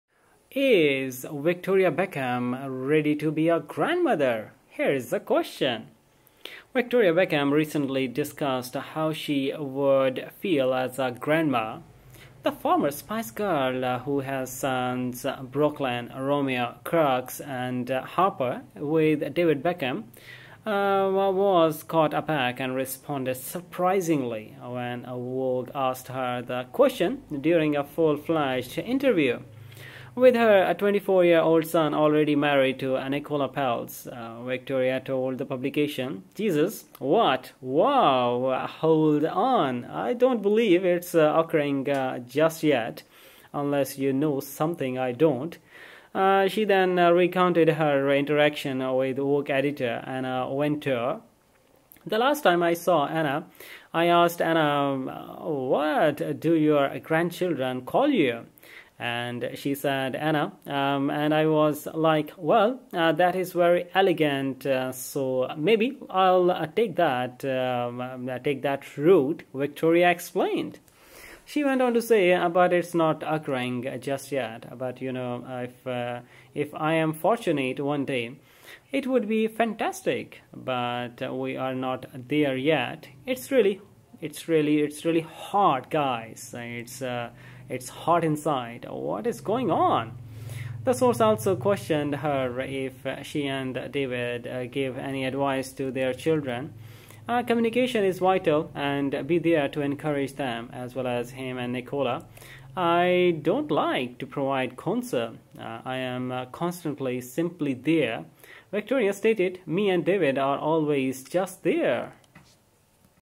Is Victoria Beckham ready to be a grandmother?
Victoria Beckham recently discussed how she would feel as a grandma.
The former Spice Girl, who has sons Brooklyn, Romeo, Cruz, and Harper with David Beckham, was caught aback and responded surprisingly when Vogue asked her the question during a full-fledged interview.
With her 24-year-old son already married to Nicola Peltz, Victoria told the publication, "Jesus." What?! Woo. Hold on. I don't believe it's occurring just yet, unless you know something I don't."
She then recounted her interaction with Vogue editor Anna Wintour.
"The last time I saw Anna, I asked, 'Anna, what do your grandchildren call you?'" And she said, 'Anna.' And I was like, "Well, that is very elegant, so maybe I'll take that route," Victoria explained.
She went on to say, "But it's not occurring just yet. But, you know, if I am fortunate one day, it would be fantastic. But we are not there yet! It's really hot, guys. It's hot inside! "What is going on?!"
The source also questioned her if she and David give any advise to their children.
"Communication is vital, and be there to encourage them, as well as him and Nicola. I don't like to provide counsel. I am constantly simply there. Victoria stated, "Me and David are always just there."